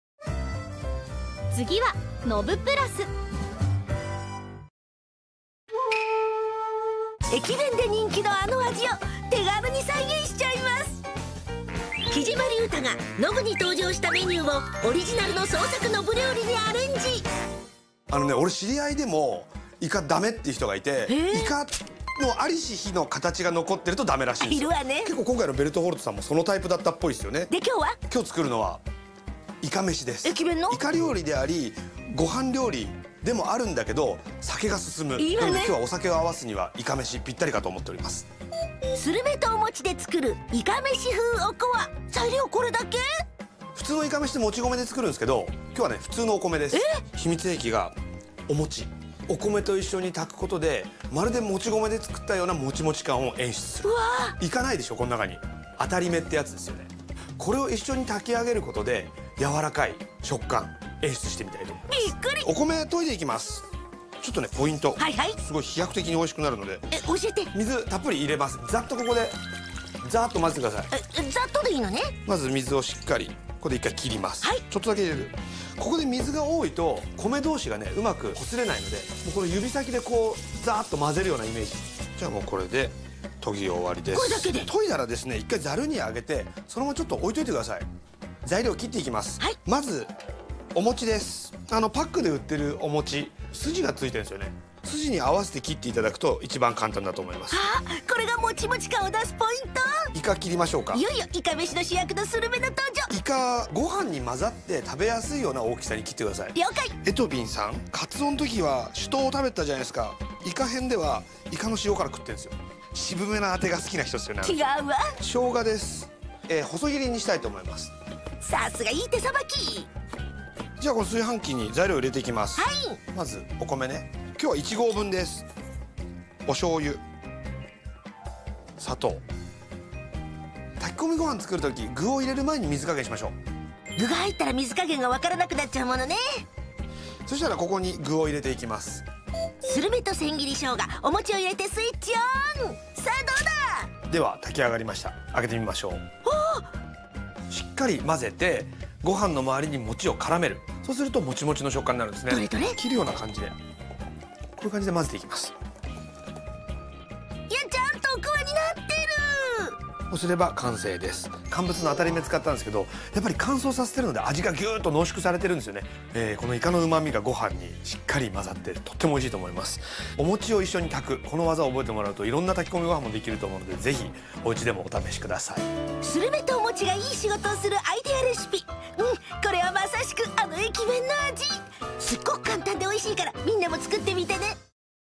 Ikameshi – Rice with Squid
Recipe from Isekai Izakaya Nobu Ep9

Ingredients
・10g dried squid (surume) (*1)
・1 rice cake (mochi) (*1)
・150g rice
・water
Sauce